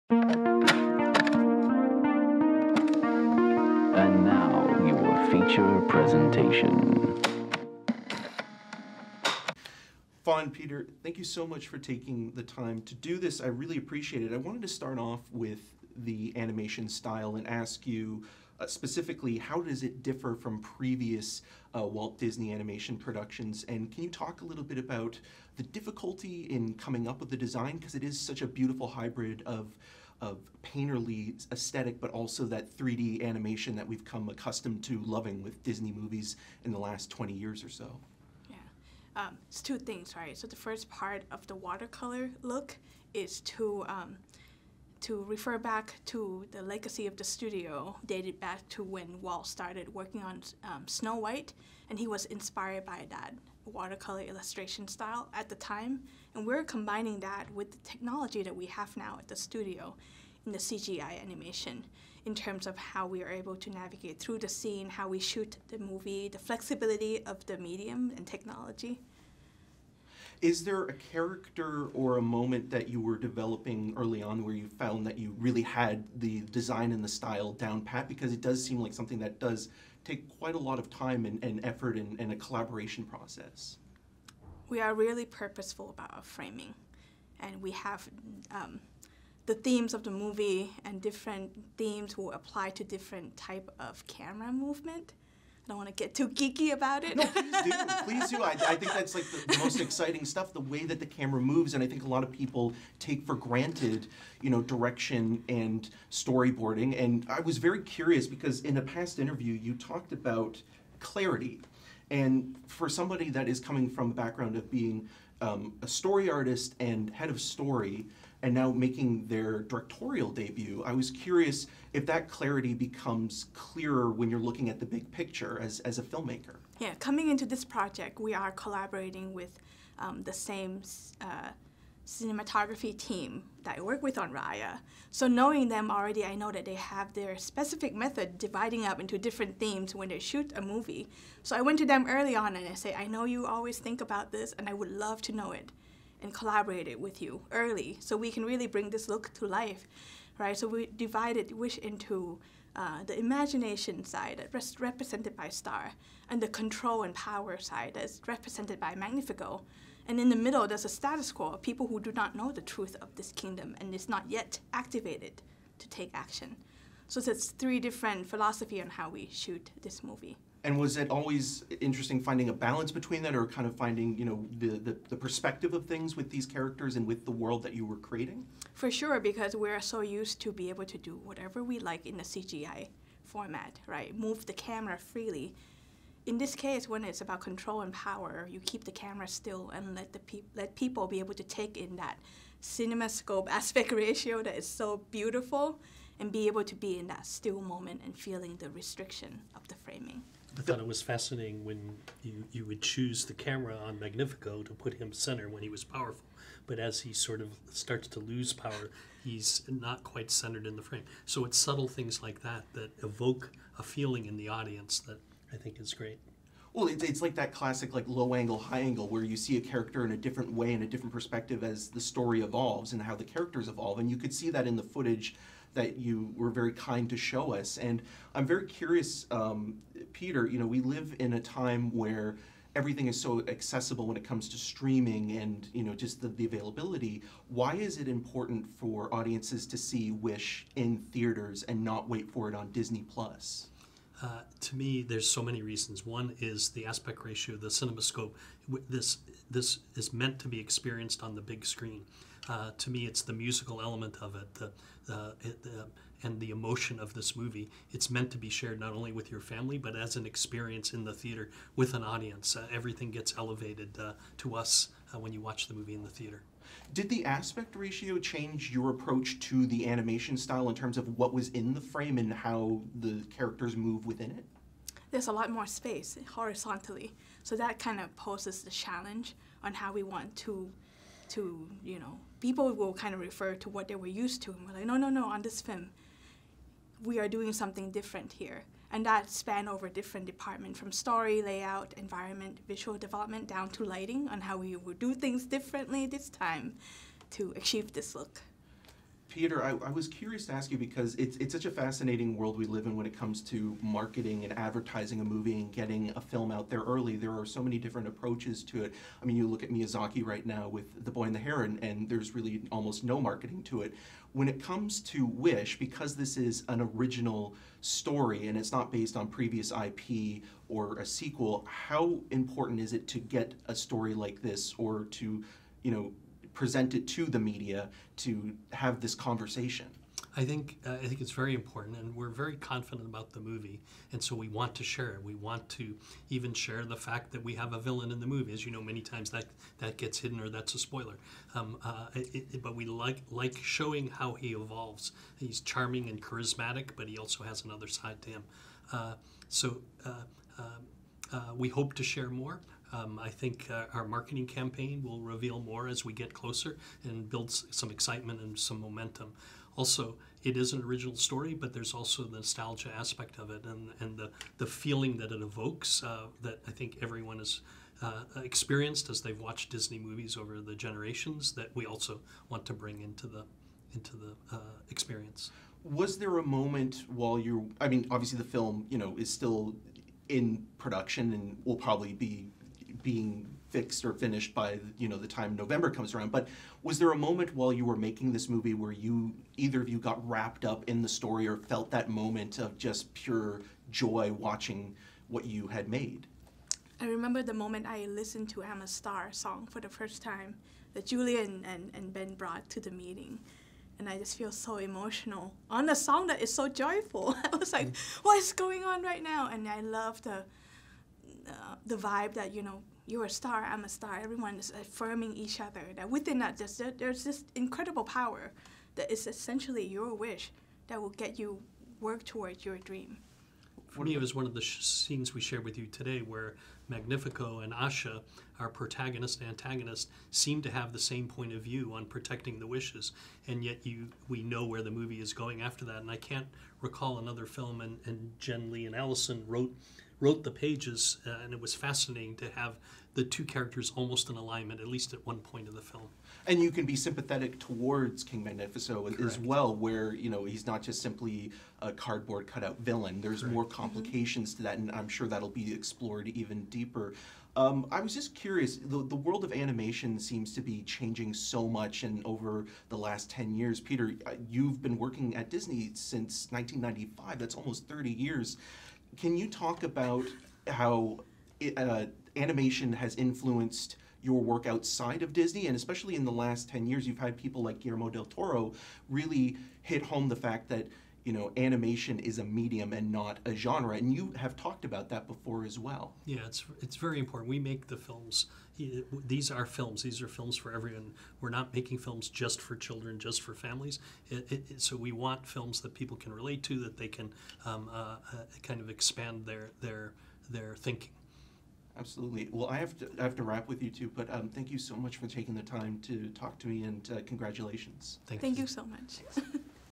Interview: Disney Wish's Fawn Veerasunthorn & Peter Del Vecho
On this very special episode of Untitled, Eric celebrates Disney 100 with Wish's Fawn Veerasunthorn & Peter Del Vecho.